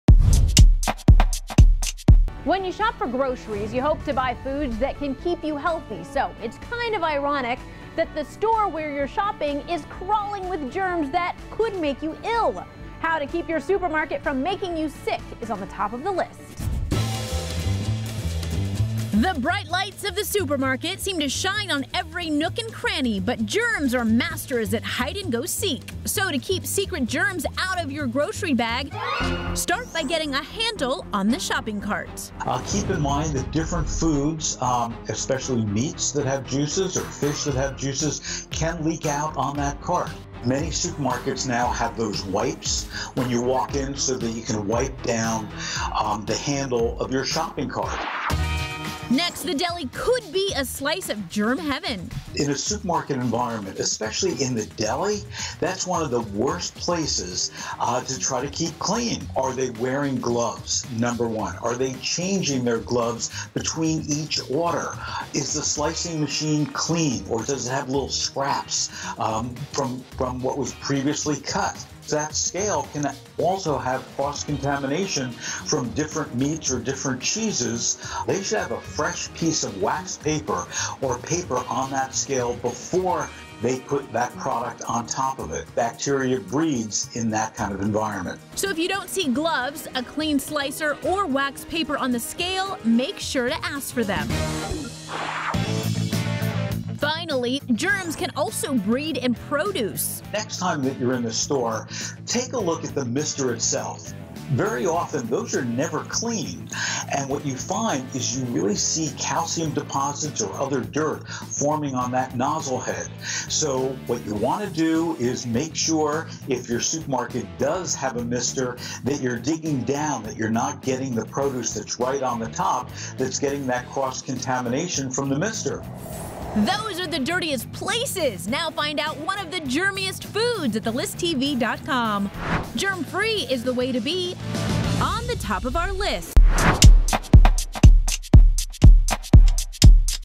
Avoid Supermarket Germs: 4 Dirtiest Foods & Places in Grocery Stores
The bright lights of the supermarket seem to shine on every nook and cranny – but germs are masters at hiding! We spoke with food industry analyst Phil Lempert to find out the dirtiest places at the grocery store – and how you can keep germs at bay!